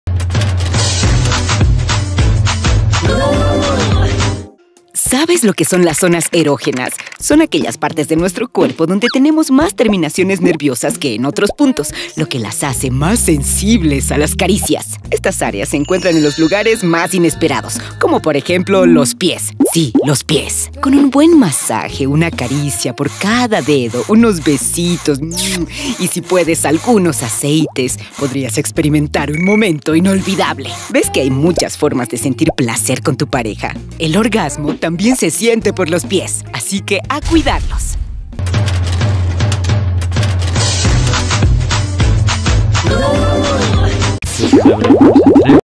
PLACERES ESCONDIDOS
Los orgasmos pueden venir de donde menos te imaginas.